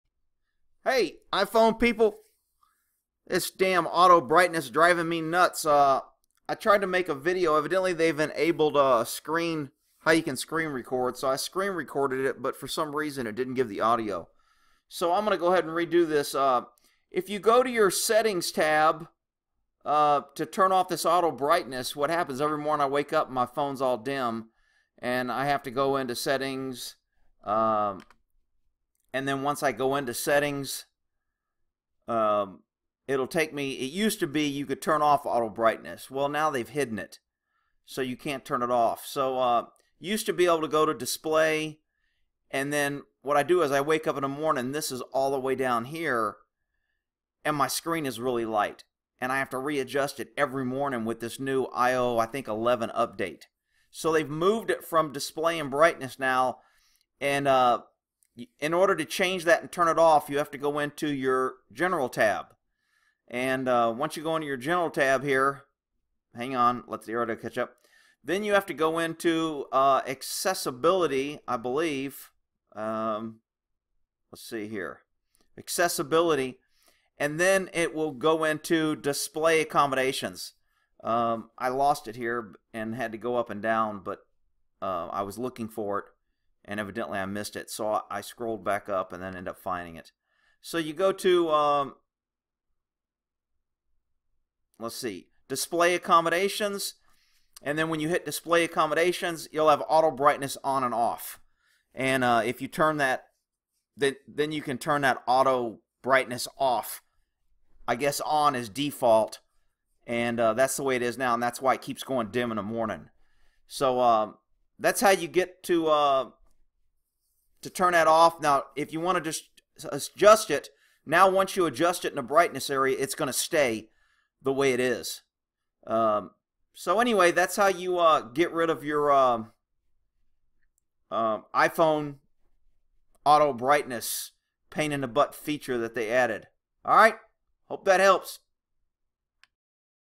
iPhone Disable Auto Brightness Feature - Newest Update IOS 11 Keeps Dimming My Phone
Here are steps to find and turn off that damn auto brightness feature.

STEPS:
Go to Settings
General tab
Accessibility tab
click on Display Accommodations
then go to Auto Brightness and turn it OFF

Thanks, hope this helps, what a pain. NOTE: MY TWITTER IS  @Horsyguy